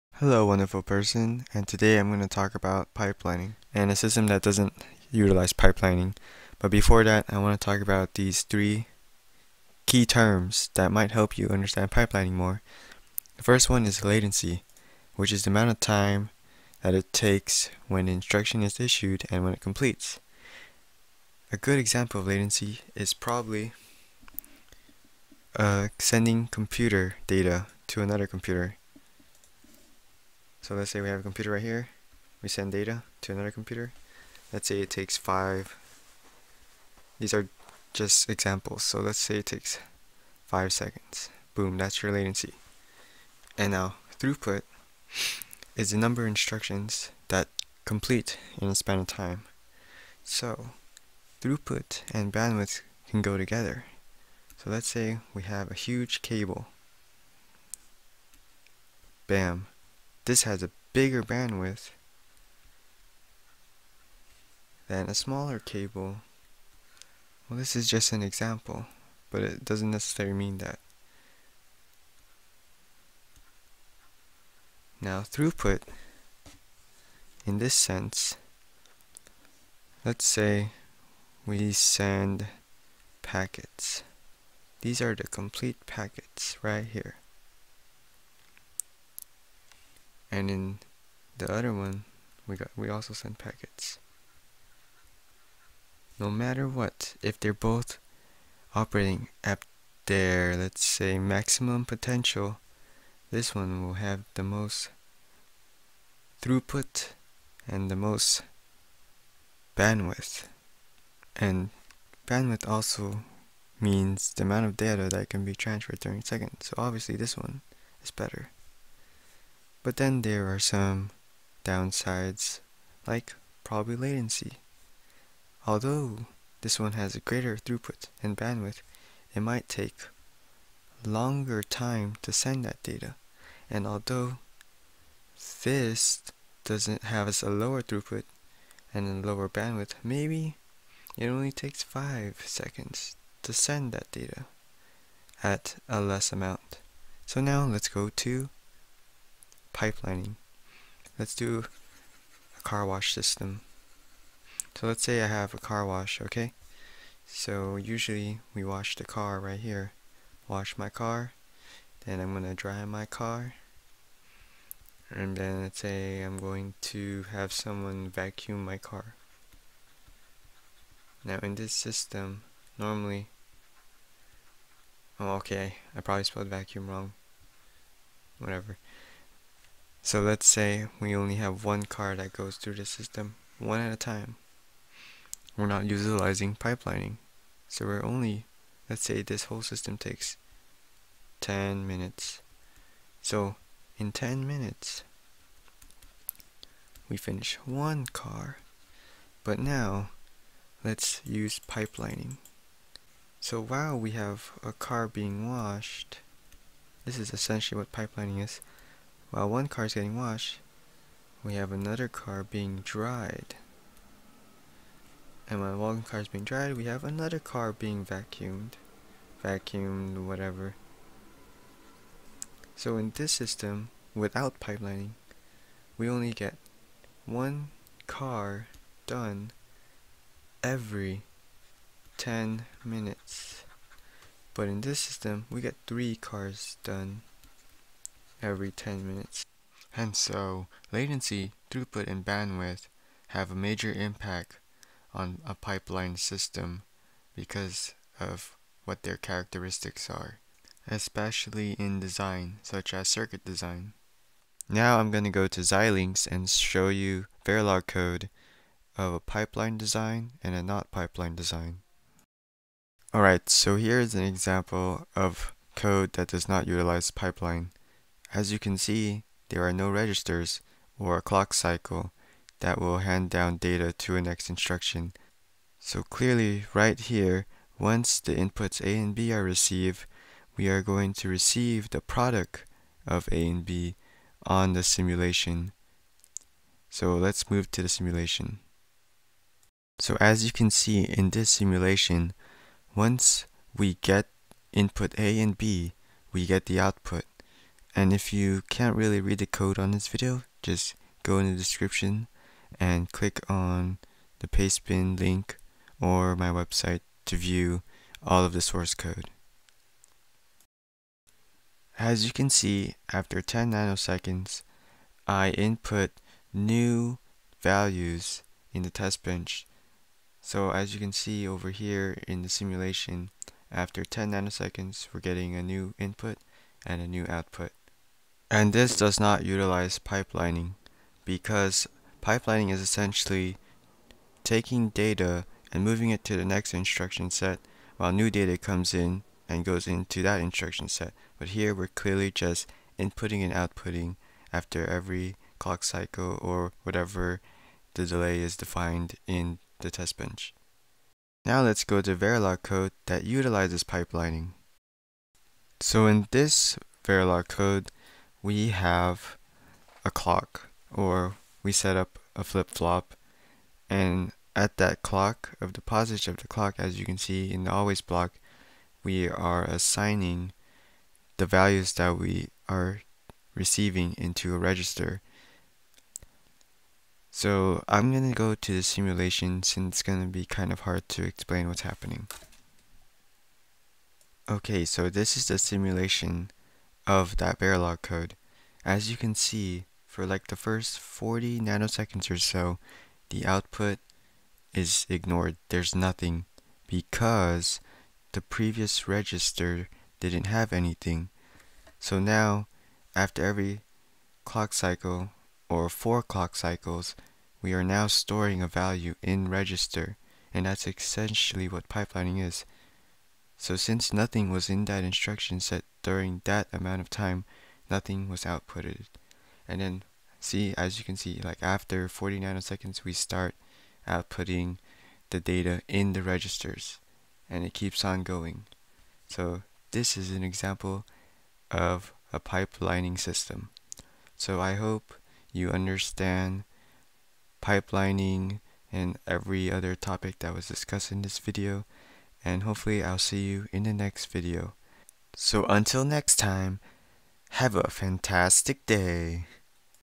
Latency, Throughput, Bandwidth, Pipelining (Verilog) Multiplier (CompEng)
Skip to 4:15 if you want an explanation of verilog code.

I wanted to make a video to help myself better understand the material by explaining it. I hope I did not tell misleading information.

You can also go to the Synthesis report to check for the timing details in Xilinx once you synthesize your design.

I didn't go in depth with verilog because that wasn't the real purpose for this video, but if you want to learn verilog:
This guy has good videos for a start: